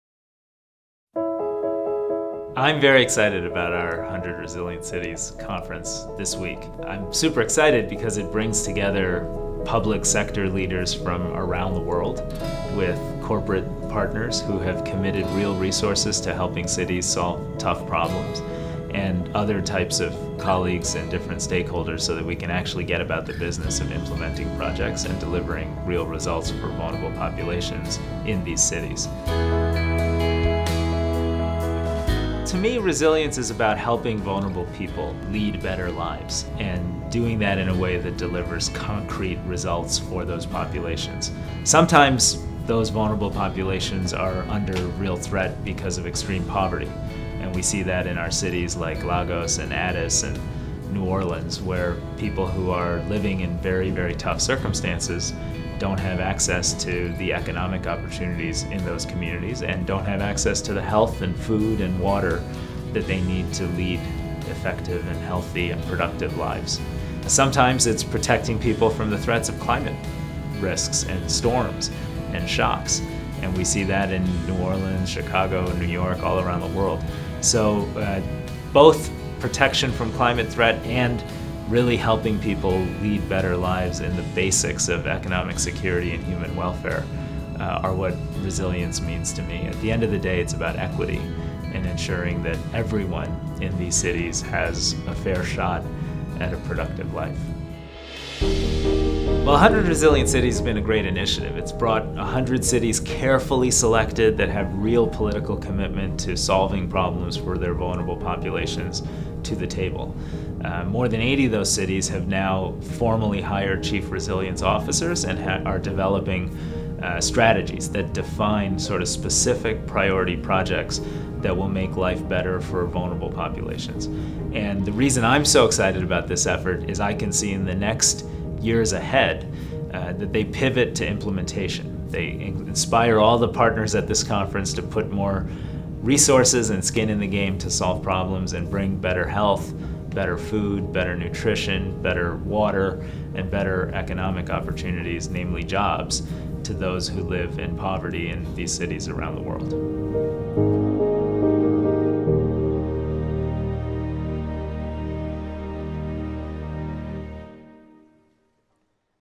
Rajiv J. Shah | Reflections on 100 Resilient Cities
This week in New York, leaders from across the global urban resilience movement came together to build solutions, establish connections, and create a path to a resilient future. But this is only the beginning. The promise of resilience lies not in what we’ve done, but what we will do going forward.